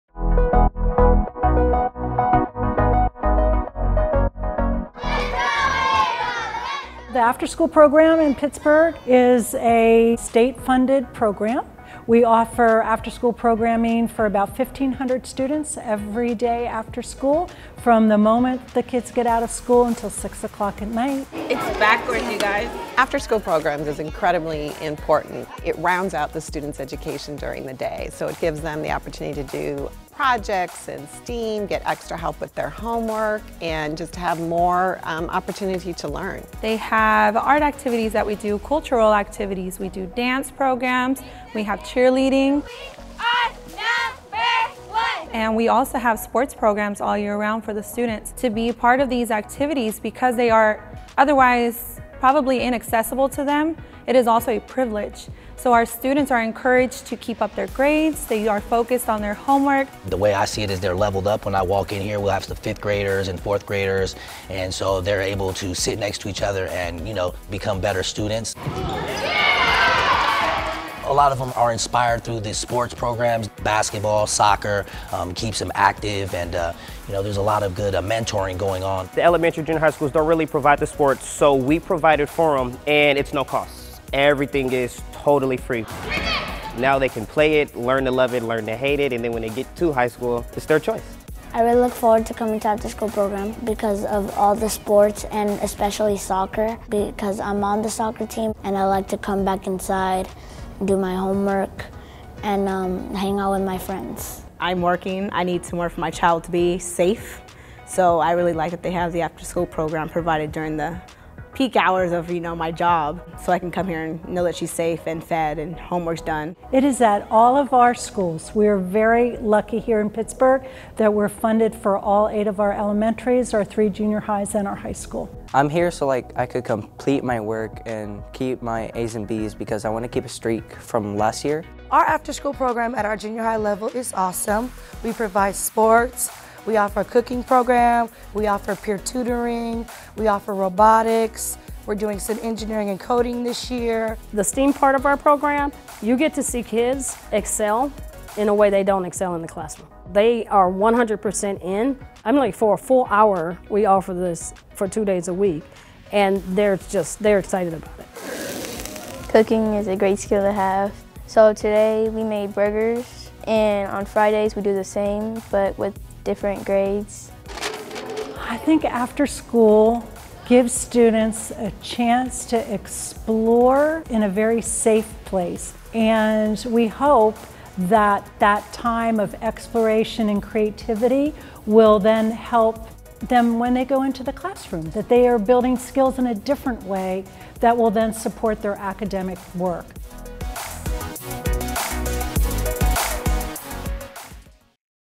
Pittsburg Unified School District's After School Program
The Pittsburg Unified School District offers no-cost After School Programs at each of our elementary and junior high sites.  Programs are available to students in 1st through 8th grades.  These programs are free to students who attend school in the district and who can benefit from this type of programming. The program starts at the end of the school day and runs to 6:00 p.m. daily. Students are expected to participate for the entire afternoon. There are limited spaces available at each site so it is best to contact the site directly to determine availability. The goal of the program is to provide a safe and healthy environment where students can engage in active learning.